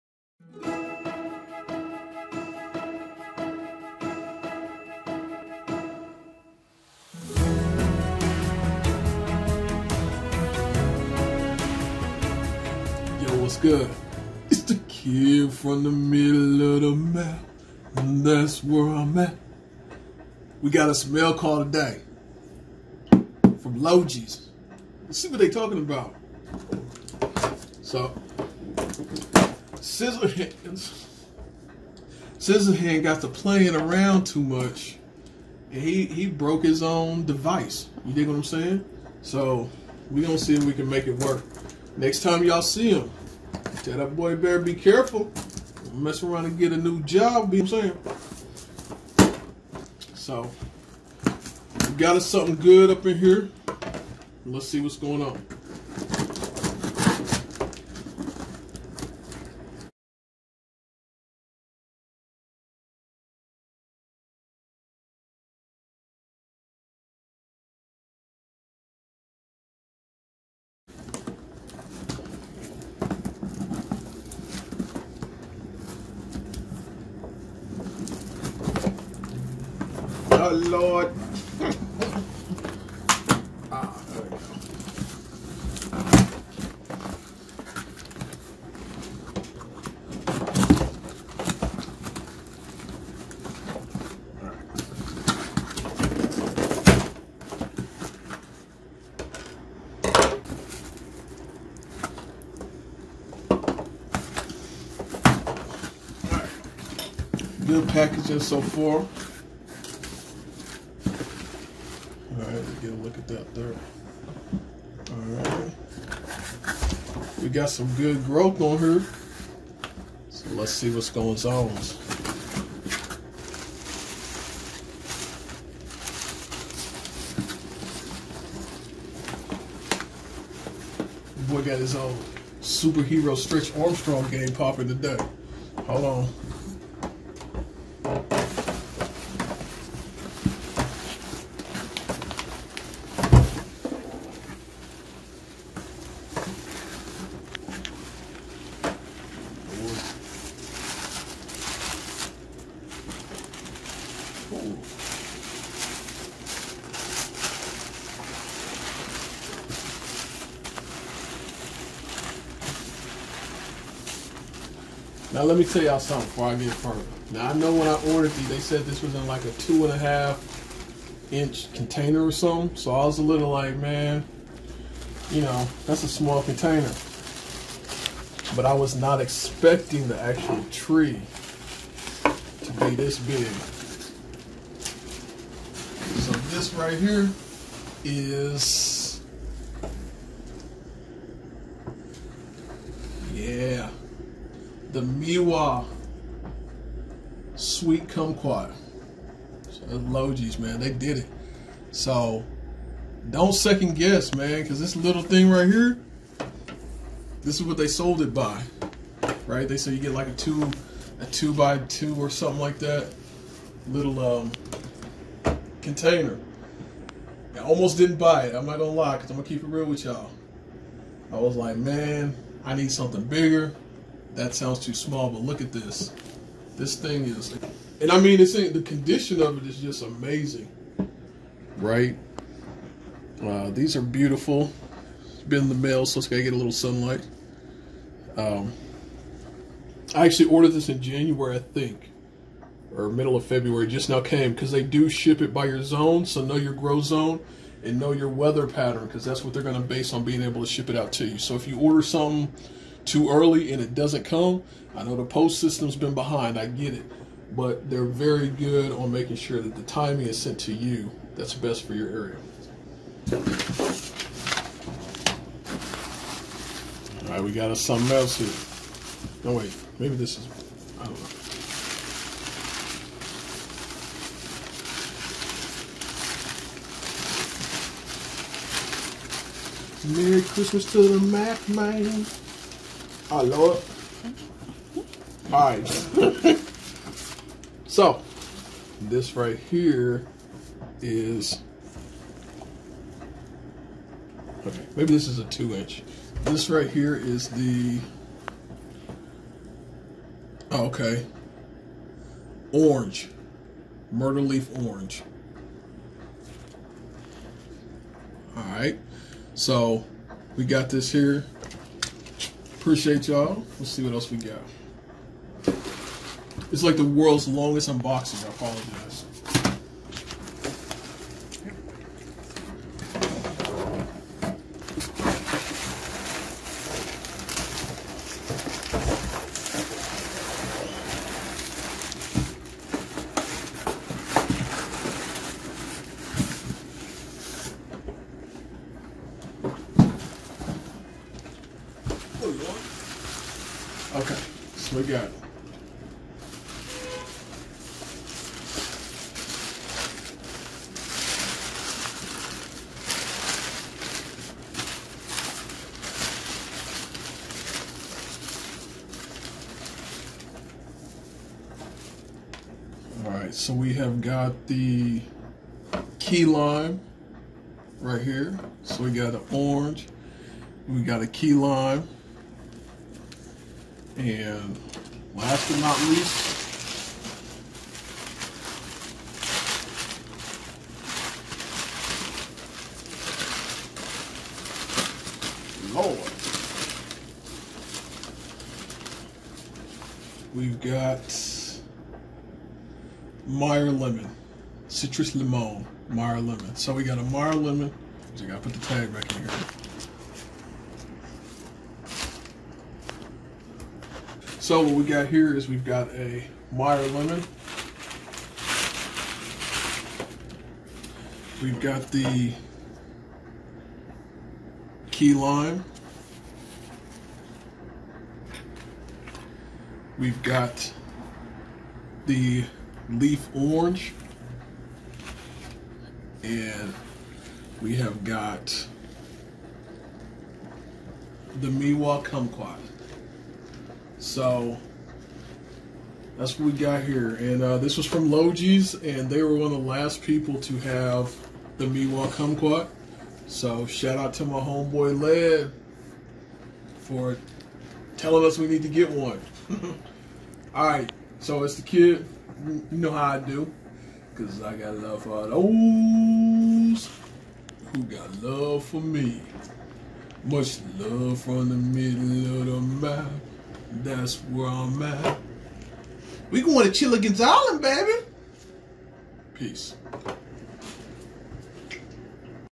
Logee's Mail Call
If you are like us, then you have probably went crazy ordering new trees and stuff for the upcoming gardening season. Well btw the crazy weather and delay in deliveries through the mail, you might have forgot that you did or what you did buy a month or two ago. That's kinda what happened to us, but now it is time to find out what the goodie man done brought us!